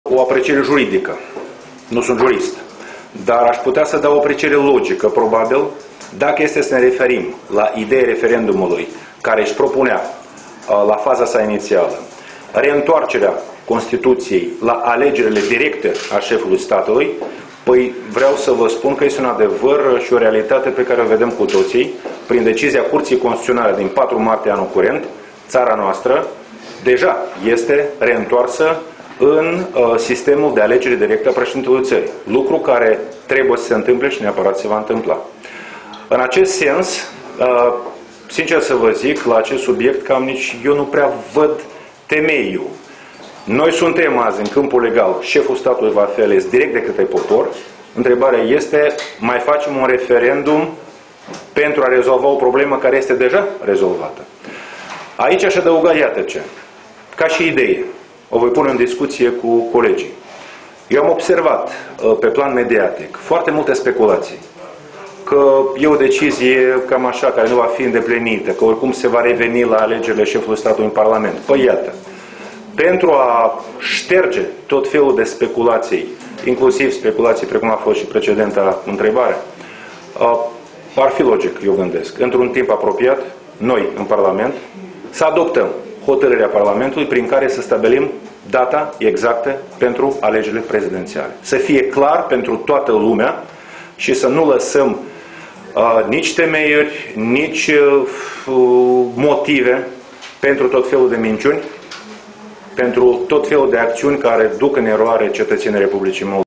Declarațiile lui Marian Lupu în timpul Ședinței Parlamentului Republicii Moldova...
Titlul evenimentului: Declarațiile lui Marian Lupu &#238;n timpul Ședinței Parlamentului Republicii Moldova din 31 martie 2016